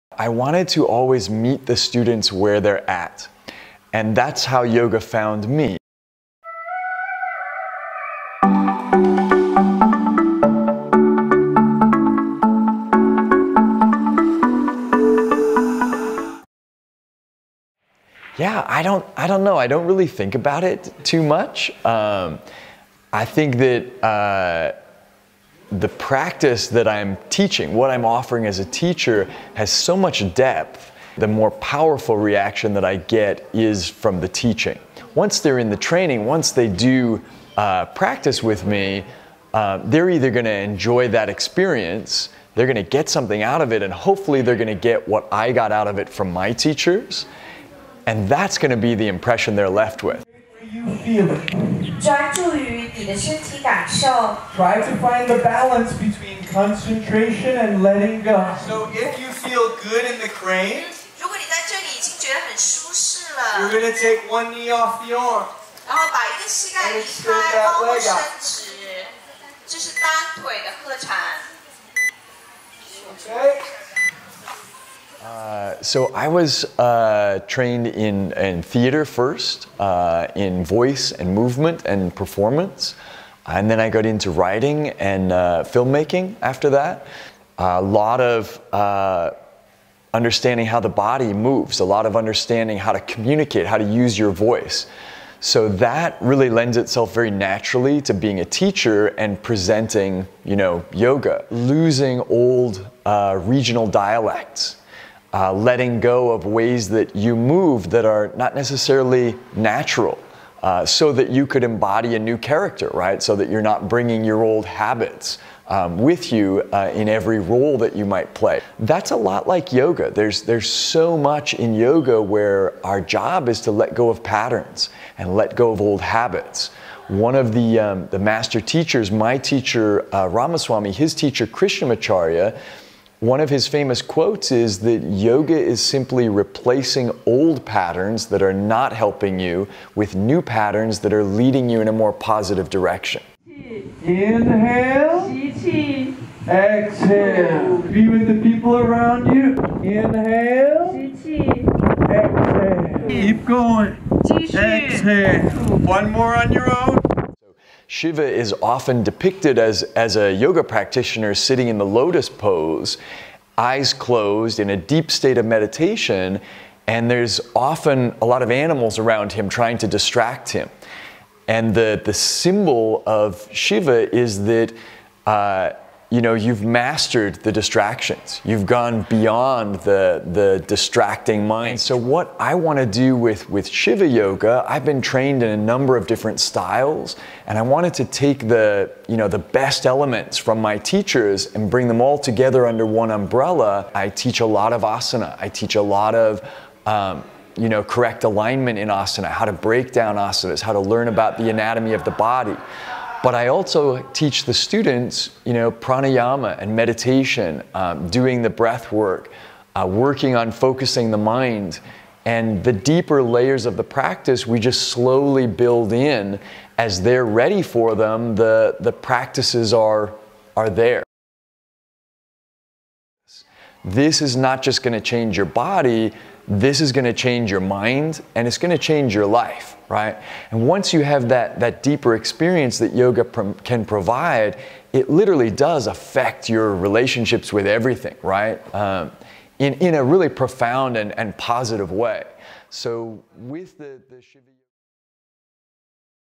Interview: Garth Hewitt - China - 2019
This short video is an interview that Garth did for the Zhejiang Yoga, Cultural & Tourism Festival in 2019 to help promote the festival and promote yoga in Zhejiang and the resort town of Shitung.

Garth talks about his background and training in the video and discusses the style and system Shiva Yoga® which he founded -- and the profound affect yoga has had on his life.

Yoga footage included with the interview is from the Zhejiang Yoga, Cultural & Tourism Festival in 2019.